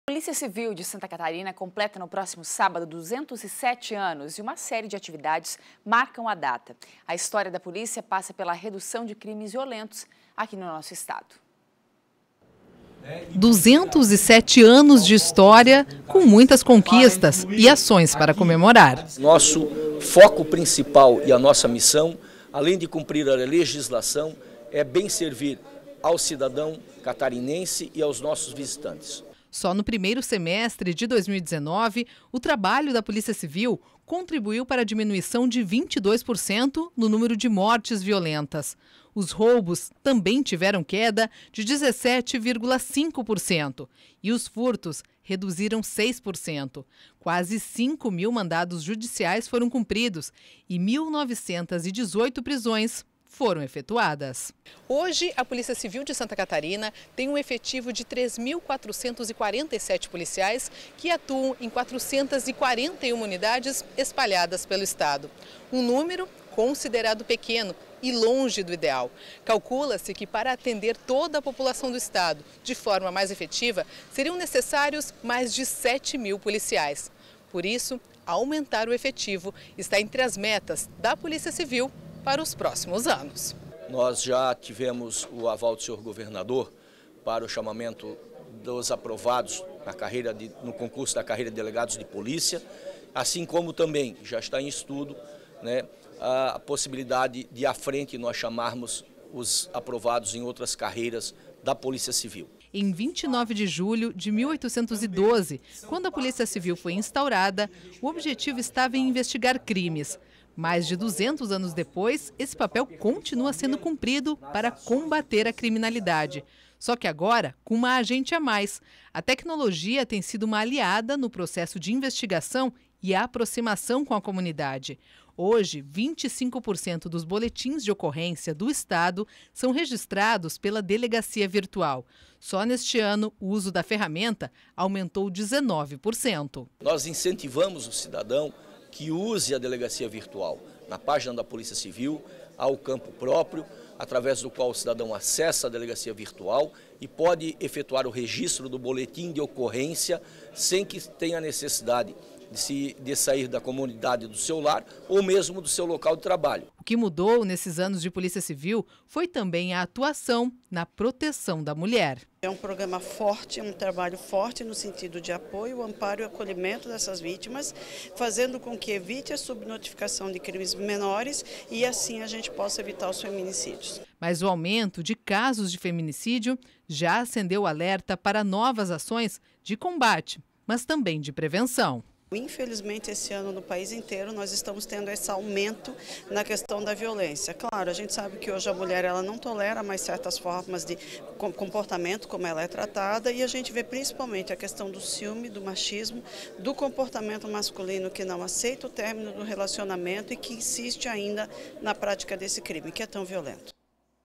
Polícia Civil de Santa Catarina completa 207 anos de atuação no estado
A Polícia Civil de Santa Catarina completa 207 anos em 2019. Uma série da atividades foi marcada para a data. A história da polícia passa pela redução de crimes violentos em Santa Catarina.

Segundo o delegado geral da Polícia Civil, Paulo Koerich, o foco maior da instituição é além de cumprir a legislação, bem servir ao cidadão catarinense e aos visitantes.

Só no primeiro semestre de 2019, o trabalho da Polícia Civil contribuiu para a diminuição de 22% no número de mortes violentas no Estado. Os roubos também tiveram queda de 17,5%, e os furtos reduziram 6%. Quase cinco mil mandados judiciais foram cumpridos e 1.918 prisões efetuadas.

Hoje a Polícia Civil de Santa Catarina tem um efetivo de 3.447 policiais que atuam em 441 unidades espalhadas pelo Estado. Um número considerado pequeno e longe do ideal. Calcula-se que para atender toda a população catarinense de forma mais efetiva, seriam necessários mais de sete mil policiais. Devido a isso, aumentar o efetivo está entre as metas da instituição para os próximos anos.

"Nós já tivemos o aval do governador para o chamamento para os aprovados no concurso da carreira de delegado de polícia. Assim como também já está em estudo a possibilidade a frente chamarmos os aprovados em outras carreiras da Polícia Civil", explica Koerich.

Em 29 de julho de 1812, quando a Polícia Civil foi instaurada, o objetivo estava em investigar crimes. Mais de 200 anos depois, esse papel continua sendo cumprido para combater a criminalidade.

Porém, agora esse combate ganhou um agente a mais, a tecnologia tem sido uma aliada no processo de investigação e aproximação com a comunidade. Hoje, 25% dos boletins de ocorrência são registradas pela delegacia virtual. Só em 2019, o uso da ferramenta aumentou 19%.

"Nós incentivamos o cidadão que use a delegacia virtual. Na página da Polícia Civil há um campo próprio através do qual o cidadão acessa a área e pode efetuar o registro do boletim de ocorrência sem que tenha necessidade de sair de casa", explica Koerich.

O que mudou nesses anos de Polícia Civil foi também a atuação da proteção a mulher. De acordo com a delegada da Polícia Civil, Patrícia Zimmermann, este é um programa forte no sentido de apoio e amparo aos sofrimentos destas vítimas, fazendo com que evite as subnotificações de crimes menores, podendo evitar os casos de feminicídio.

O aumento de casos de feminicídio já acendeu o alerta para novas ações de combate e prevenção. "Infelizmente esse ano no país inteiro estamos tendo esse aumento em questão da violência", lamenta Zimmermann. "Sabemos que hoje a mulher não tolera mais certas formas de comportamento, de como ela é tratada, e a gente vê principalmente a questão do ciúme do marido, do machismo e da não aceitação de términos de relacionamentos", explica a delegada.